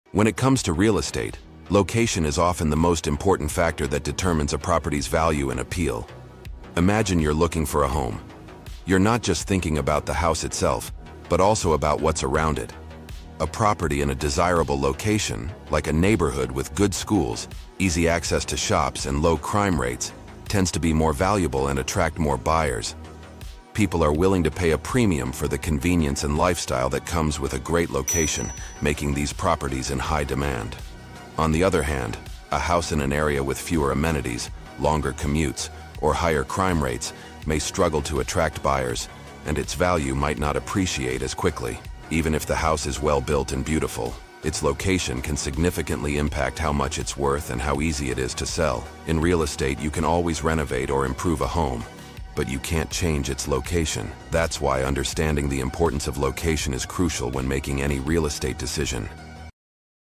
Why Location is Everything?
When buying a home, location is key. A property in a desirable area—near good schools, shops, and low crime rates—can command a higher price and attract more buyers. While you can renovate a house, you can’t change its location, so choosing wisely is essential.